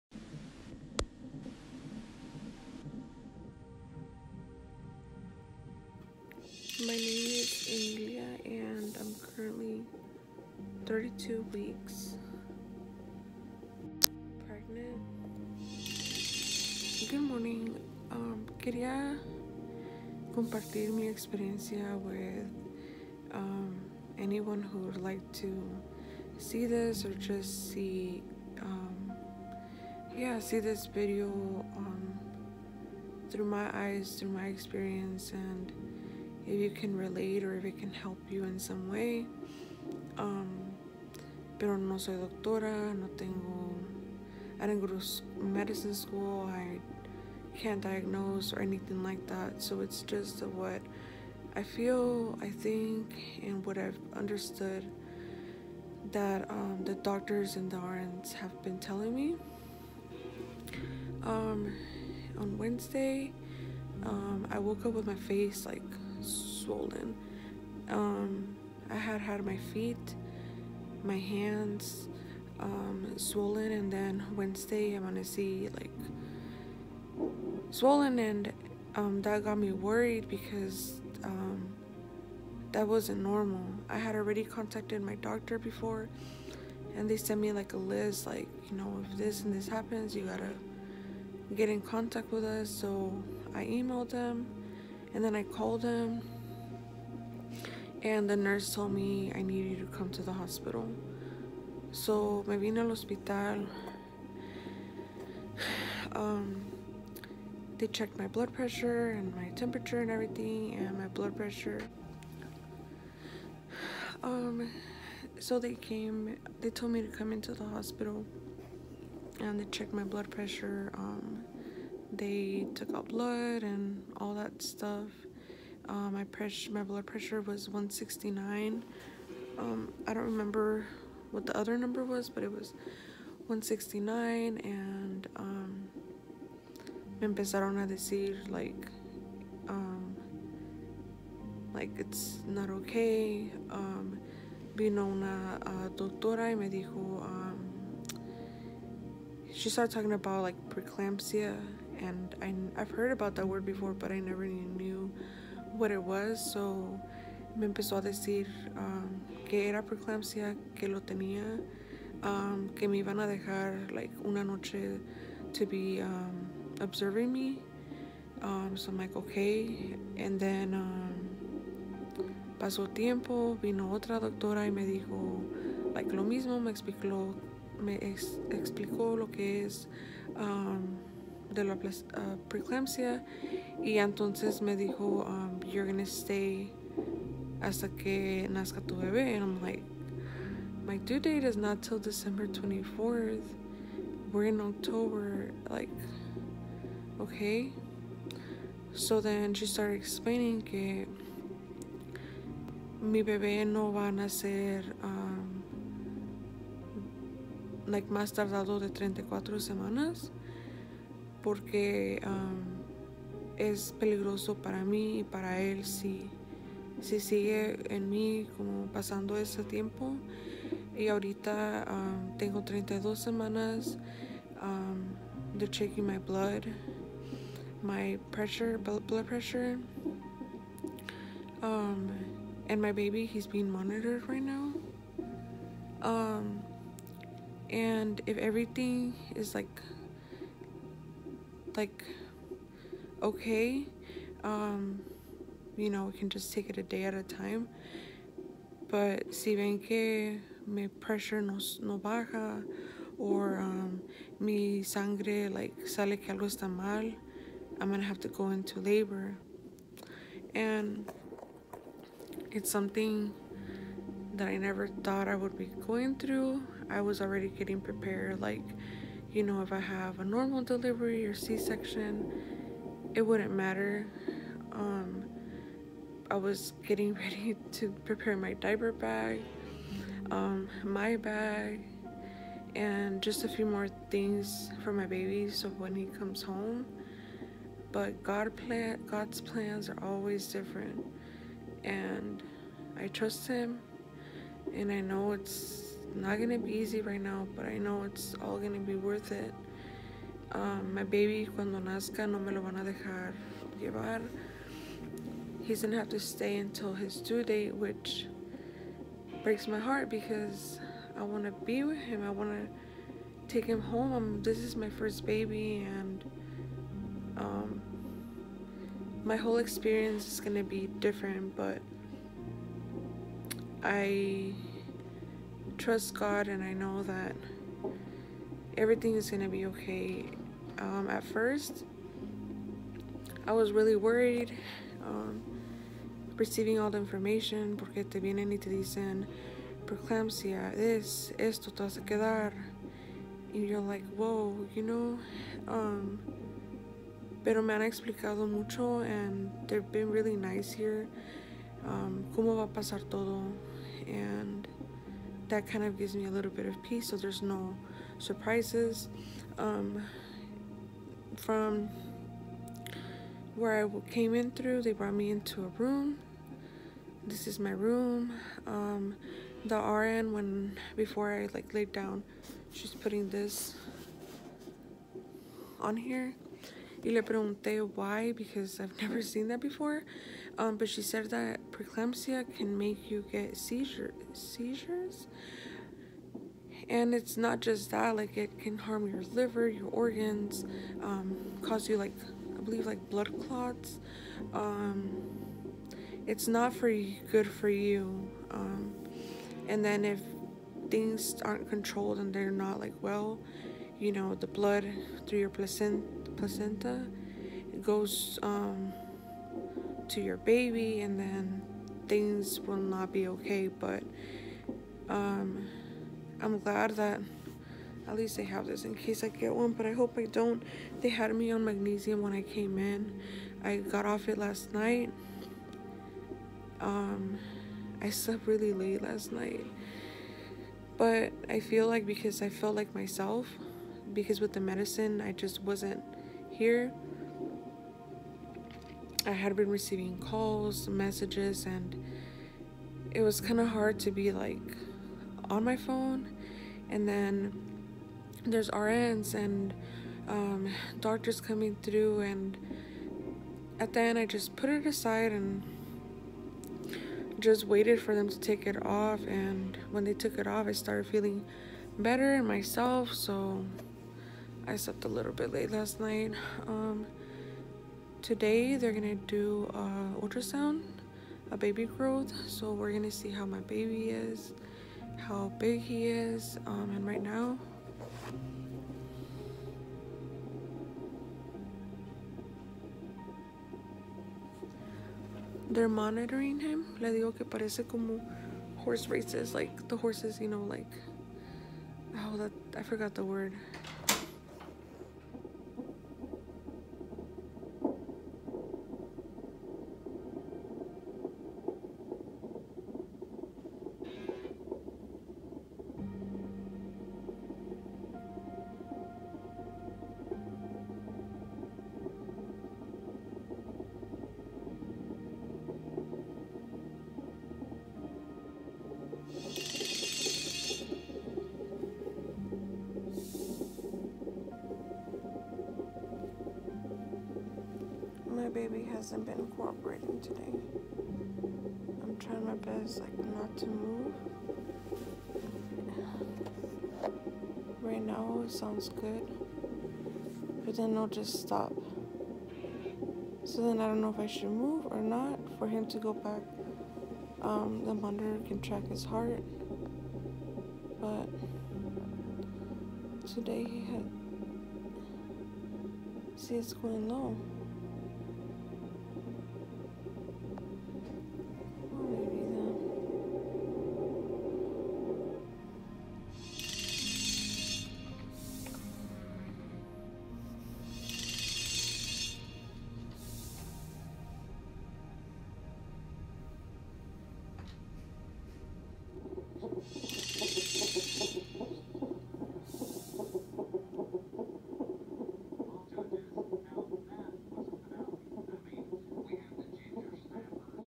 My hospital stay due to preeclampsia at 31 weeks
I had no idea I had preeclampsia, never really knew what it was until I didn’t get to go home because of it, and I am still learning about it. My baby was born at 32 weeks and 5 days.